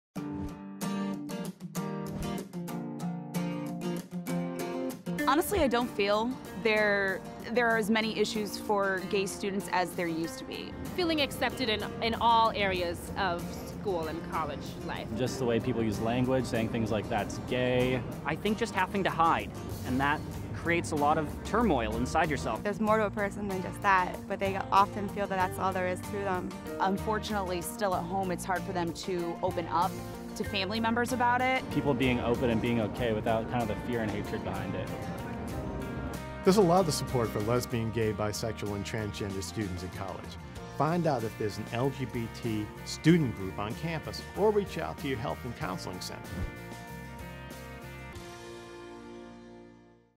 Issues Gay Students Might Face | Aetna
College students share what issues they think gay students face in college.